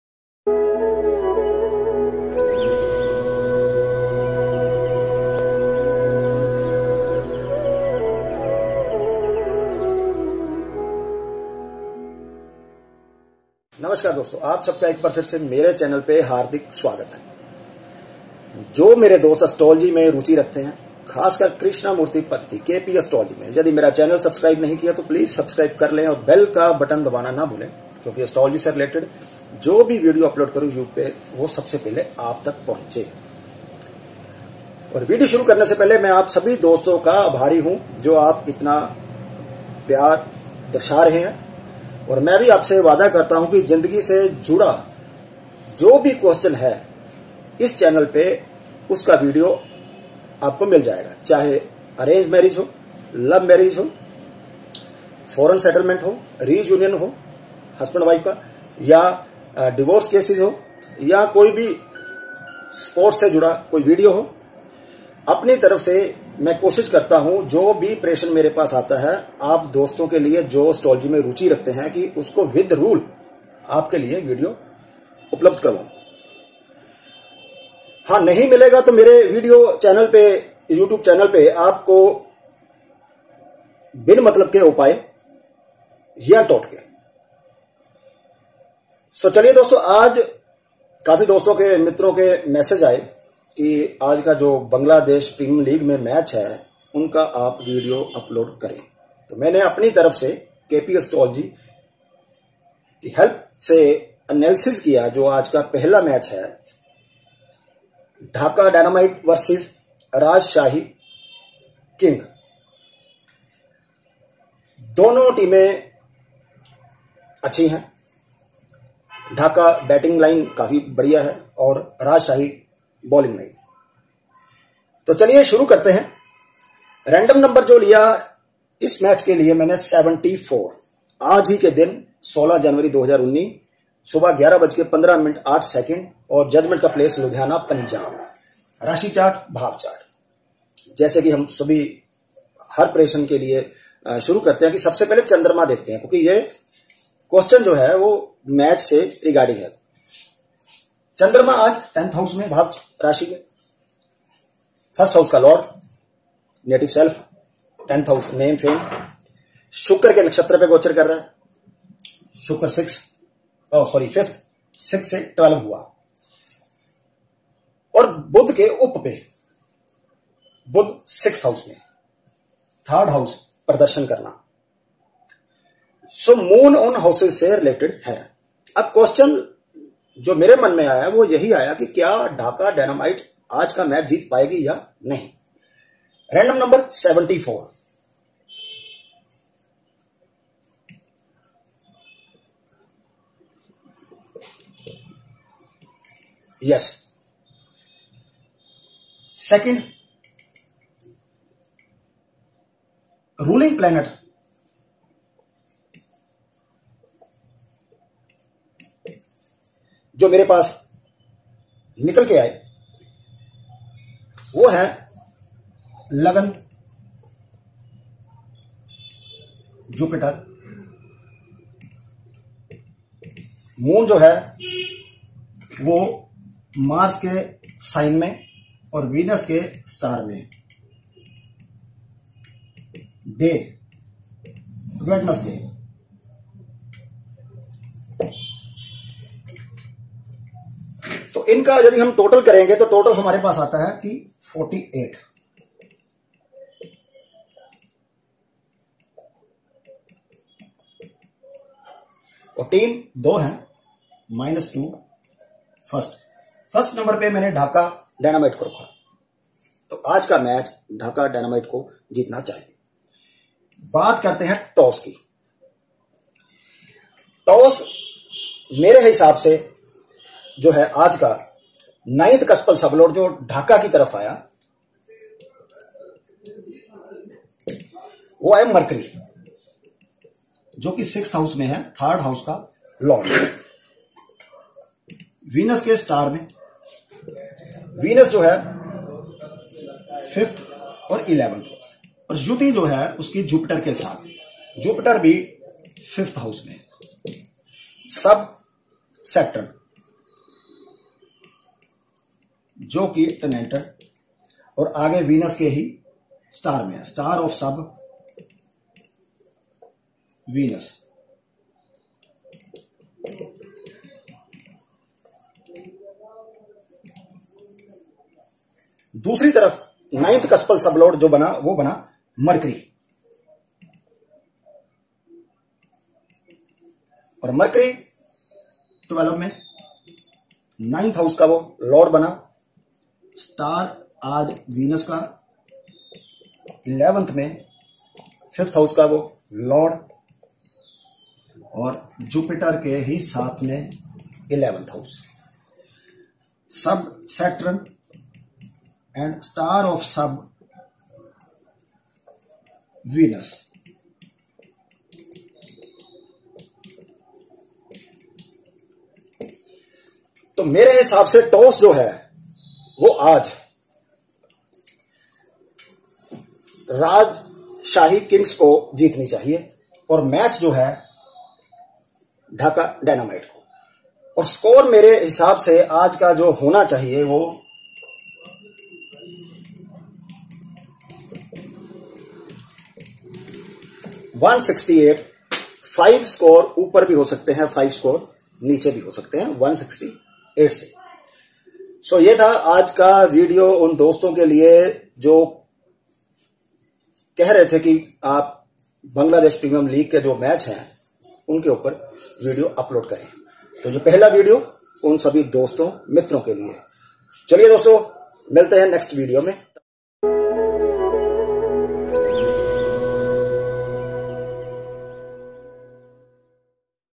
Cricket match prediction through astrology | Who will win the match in astrology | Sports astrology
Cricket match prediction through astrology | Who will win the match in astrology | Sports astrology, BPL 2018-19, 17th match, Dhaka Dynamites V/s Rajshahi Kings

Learn KP Astrology
only in 7 days